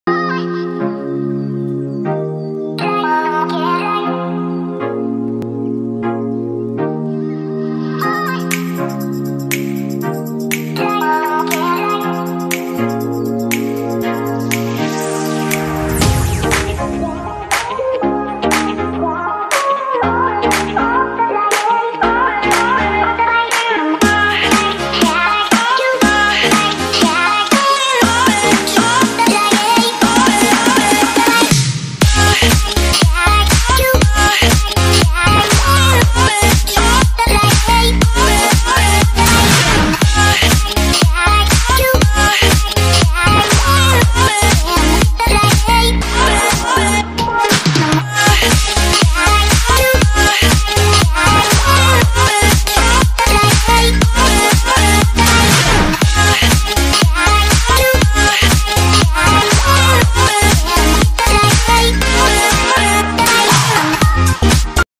What people trade for Golden Egg!!
Omg I finally got my golden egg from my streak let’s see what people offer!